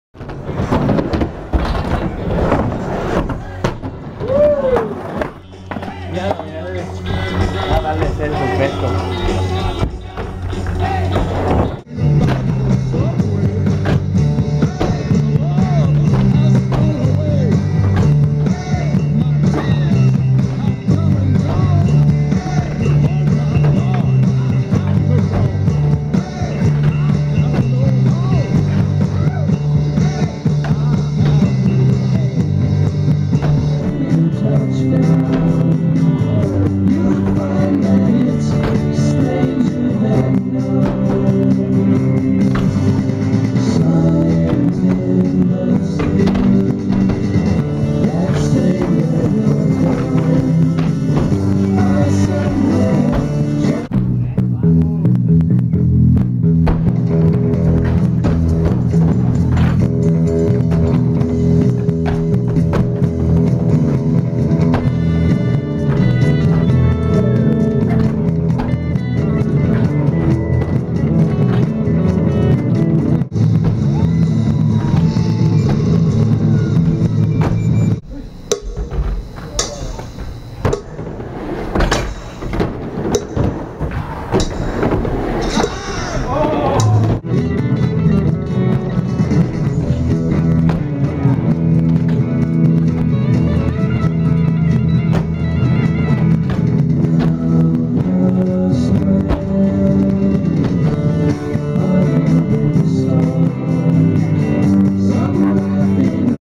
Mini Ramp sess "atardecer perfecto"
Mini Ramp sess "atardecer perfecto" Feat @Alexis Rivera @Pedro Huck Rosa , @Orlando Ramos @ Wilmer Gonzalez
 blastin good times ripping  with the Federación de skateboarding de PR. crew!!

Filmed & edited by : @wilmerfilmer